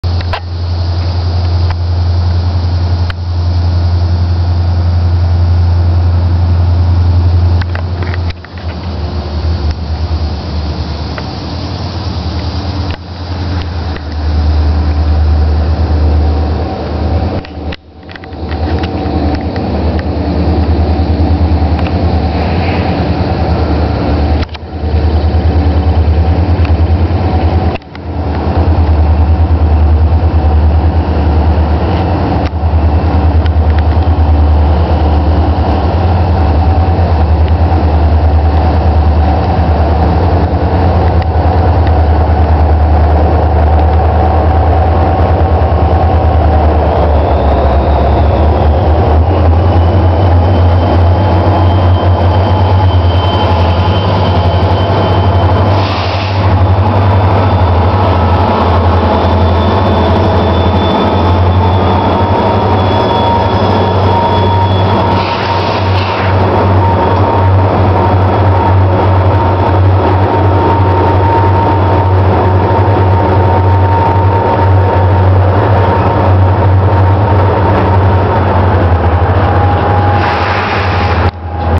3 Ex BN SD40-2's and a Ex BNSF SD40-2 in the trees
4 SD40-2's in run 8 head southbound with HLCX orange (Ex BNSF) as the 3rd unit. It is a nice lashup! I know it is not a good shot, because we were at my grandma's and I just happened to hear a faint RS3K.

9-23-08